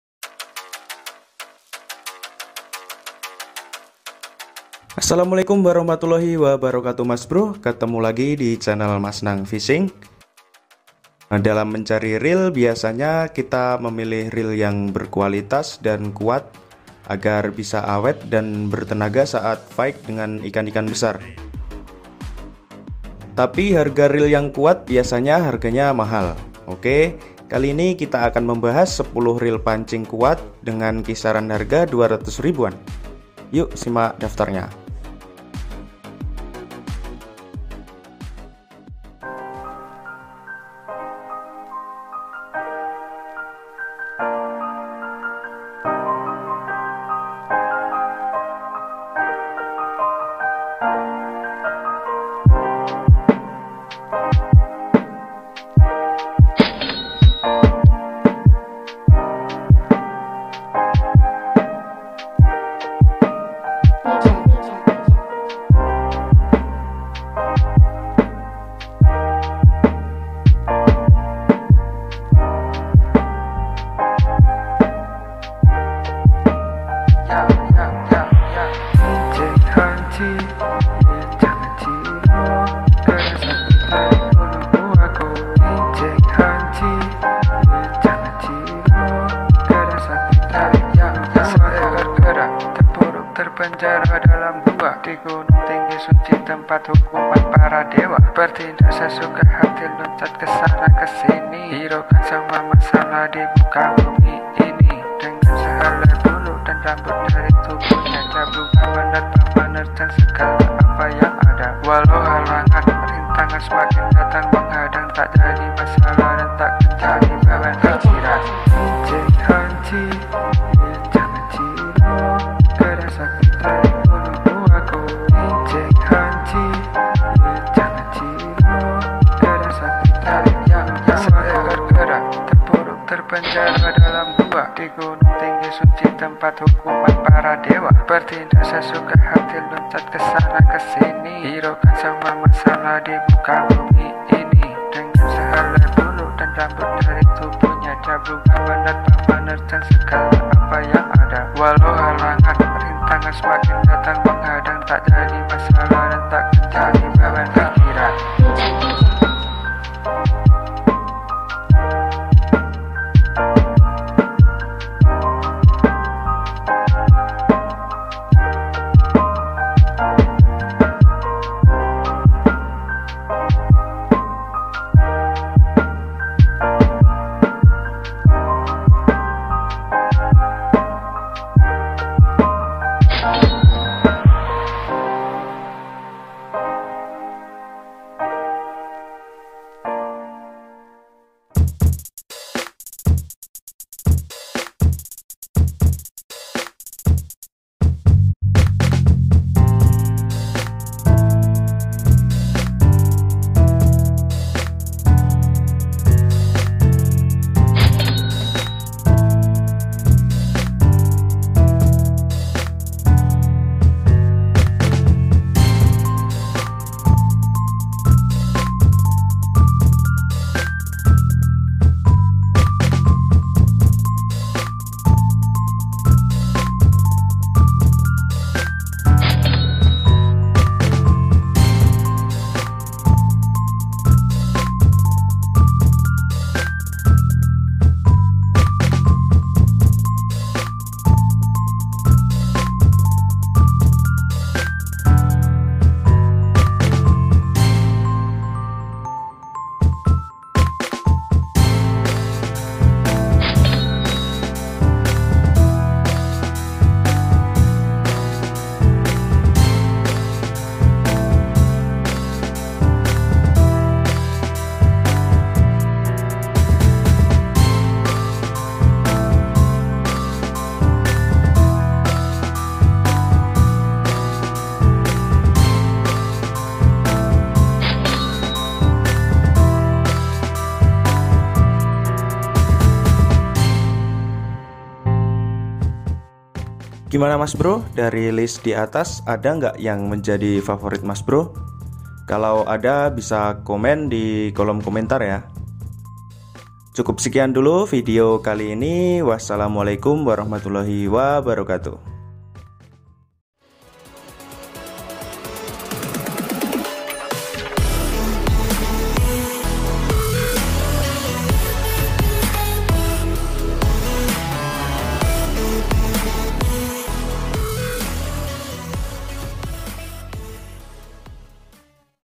10 Reel Pancing Harga 200 Ribu-an Murah Tapi Kuat
Bertemu lagi di channel Mas Nang Fishing. Dalam mencari reel biasanya kita memilih reel yang berkualitas dan kuat agar bisa awet dan bertenaga saat fight dengan ikan" besar. Tapi harga reel yang kuat bisanya harganya mahal. Kali ini kita akan membahas 10 reel pancing kuat dengan kisaran harga 200 ribuan, yuk simak daftarnya.

1. Maguro Barra 1000
2. Maguro Barra 2000
3. Golden Fish Metanium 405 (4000)
4. Golden Fish Metanium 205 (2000)
5. Golden Fish Metanium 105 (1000)
6. Swan G-Dragon 2000
7. Exory Acura 2000
8. Captain Taurus 6000
9. Versus Coleman 1000
10. Versus Duval 3000

Gimama mas bro , dari list diatas ada nggak yang jadi favorit mas bro? kalau ada bisa komen dikolom komentar.

Sumber Jejaksemut,com (, ganti .)